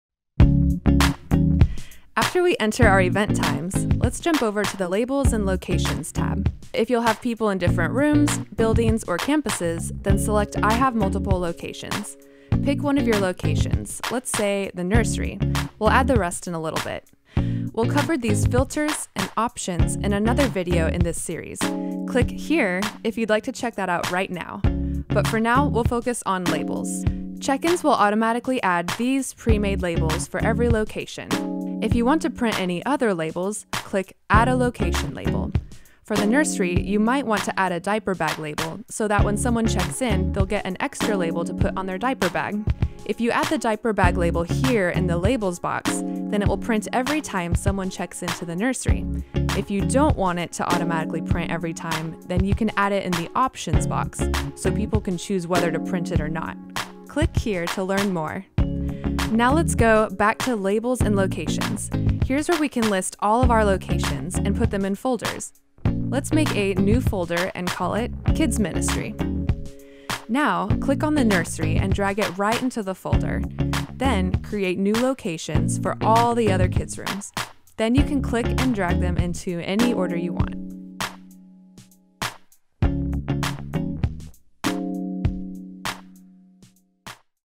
Labels & Locations in Planning Center Check-Ins
After you enter your event times, jump over to the Labels & Locations tab.

For more info on setting up locations, check out this article

This is Part 3 in our series on getting started in Planning Center Check-Ins.